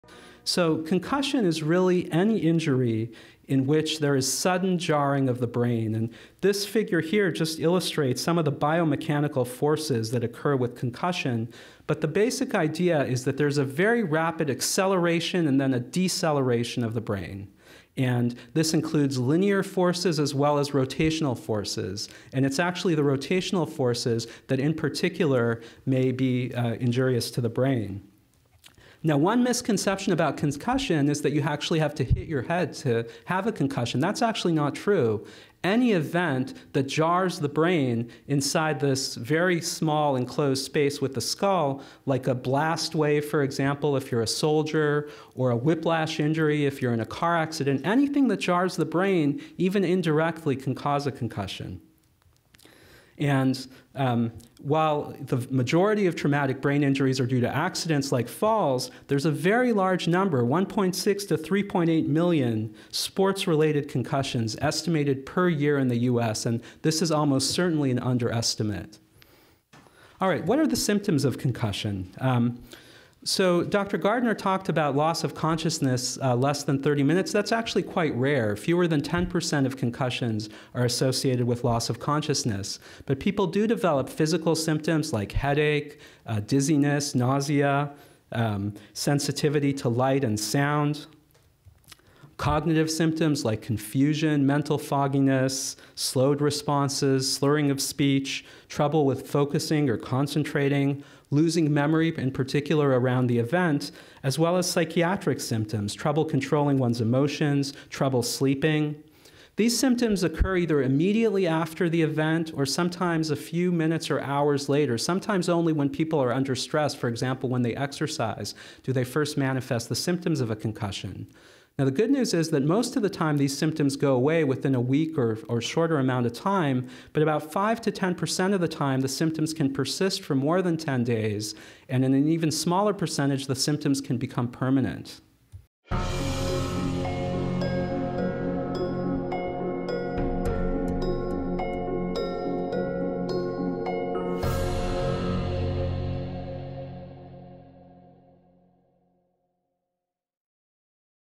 What is a Concussion?
Dr. Gil Rabinovici shares the details and definition of the oft-heard brain injury known as concussion. Series: "UCSF Osher Center for Integrative Medicine presents Mini Medical School for the Public" [Show ID: 33039]